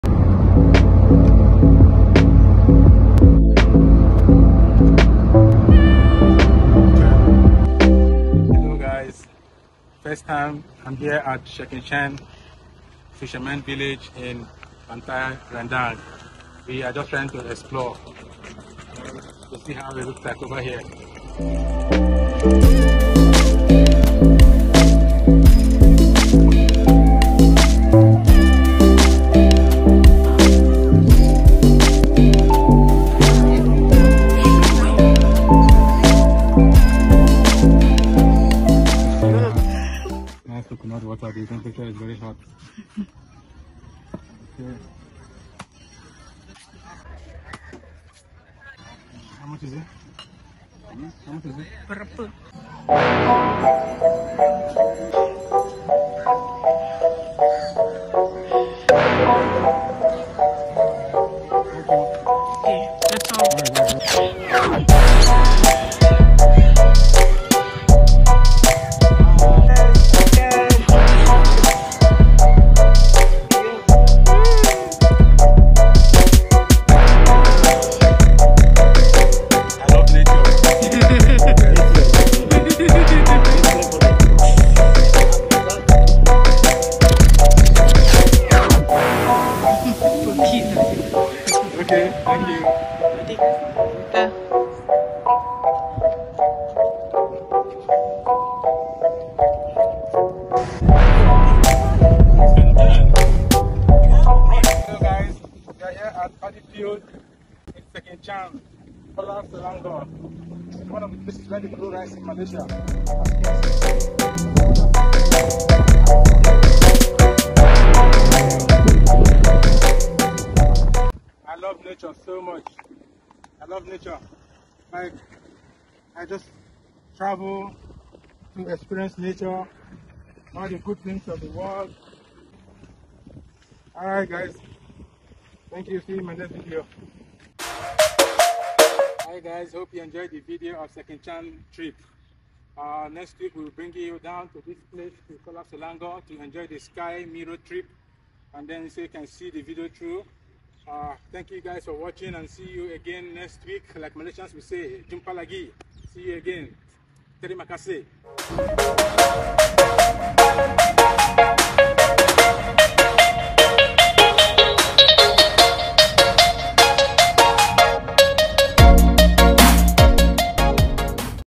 Thrilling Adventure at Sekinchan Beach Pantai Redang | Paddy Field Kuala Selangor
✅ Thrilling Adventure at Sekinchan Beach Pantai Redang | Paddy Field Kuala Selangor lets you join me for an exciting time at Sekinchan Beach, also known as Pantai Redang, and the nearby paddy fields in Kuala Selangor.

👉 Best adventure playlist link ☝️ Check out the playlist above